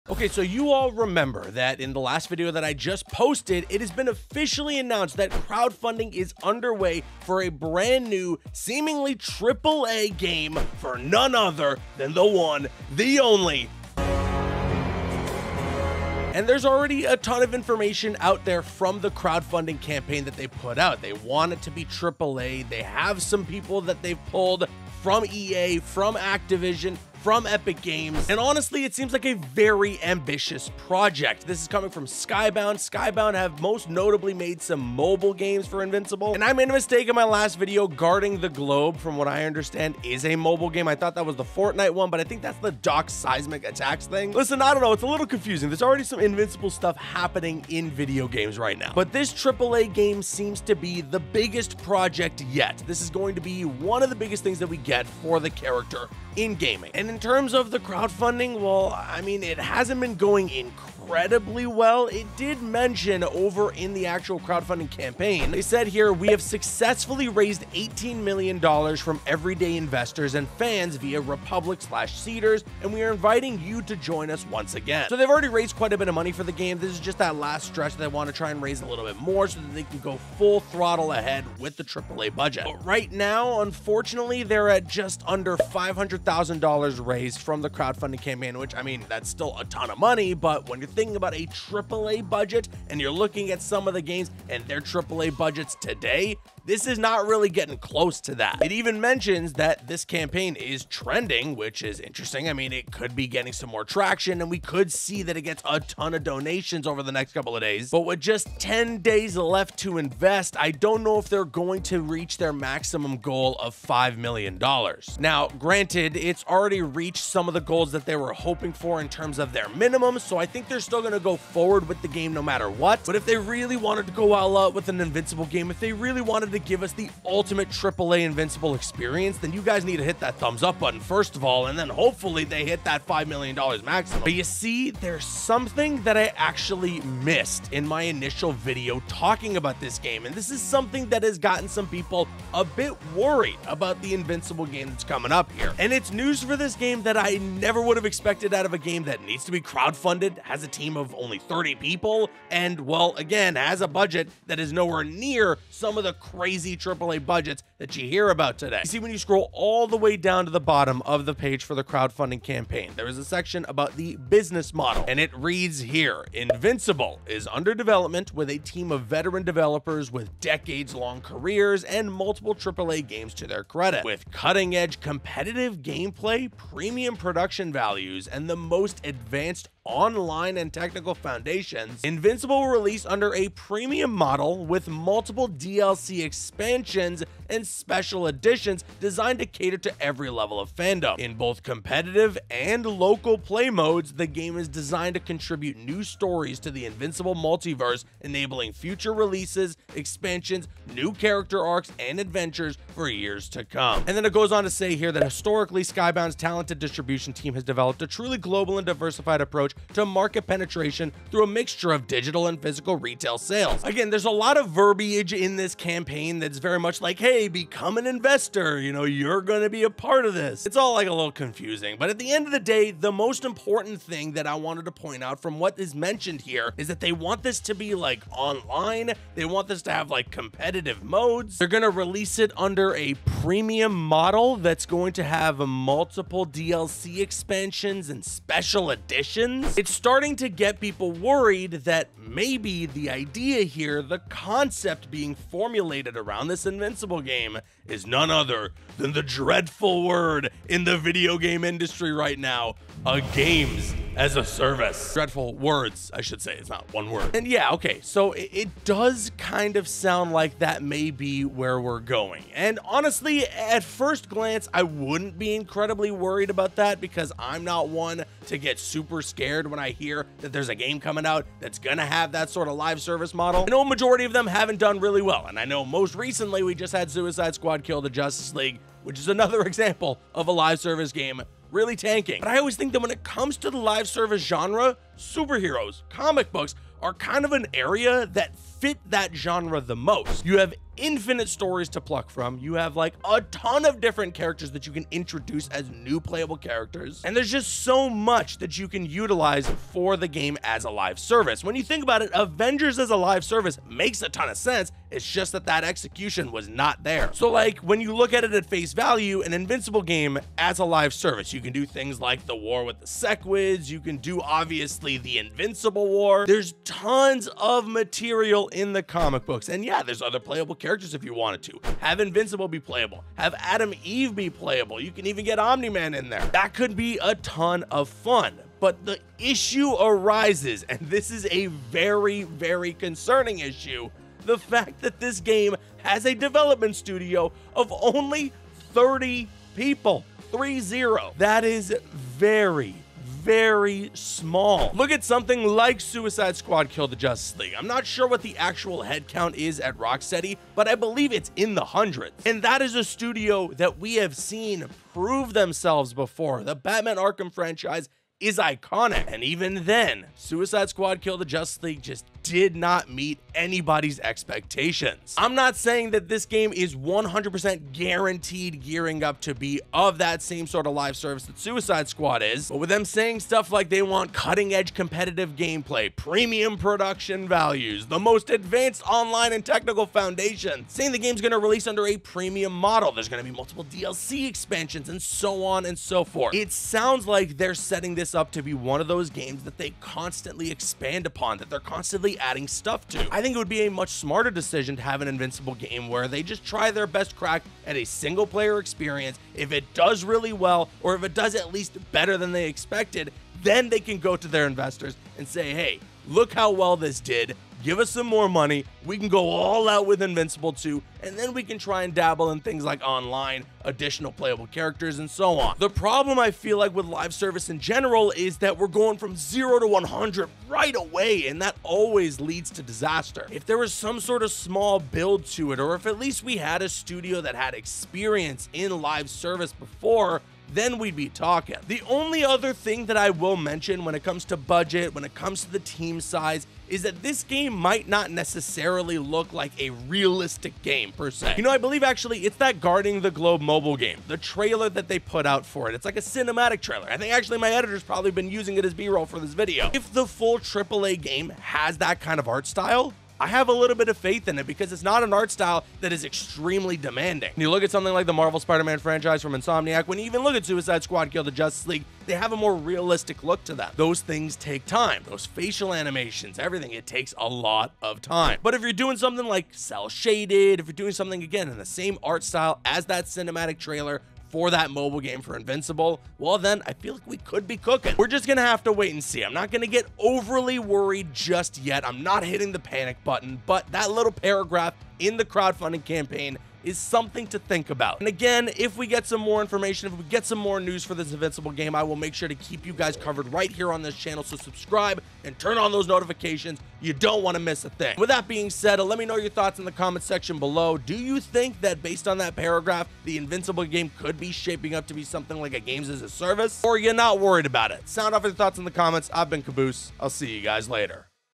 The NEW Invincible Game Just Got Bad News...
The NEW Invincible Game Just Got Bad News... With the recent news that a AAA Invincible Game is in the works, people were quick to find something that may be a little concerning about the games development. I break all that down in today's video.

ABOUT THE INVINCIBLE GAME:
By developing this game in-house, under complete control of our team of franchise stakeholders and Invincible IP creator Robert Kirkman himself, we are creating a highly coveted game that is guaranteed to have fans of the comic, the global hit TV show, and serious gamers alike, wholly captivated by this first-of-its-kind Invincible experience. Plus, by creating this project in-house, we will reap the benefits of being the IP rights owner, developer, and publisher, providing increased value to our stakeholders.

You’ve never seen a game like this from Skybound before. We have a talented team of more than 30 experienced employees from the likes of Activision Blizzard, Electronic Arts (EA), Epic Games, and more as the driving force behind our in-house studio. The studio will be headed by newly appointed Head of Games Publishing, Chris Paulson (formerly of Activision Blizzard), and EVP of Games Production, Patrick Gilmore (formerly of Amazon Games).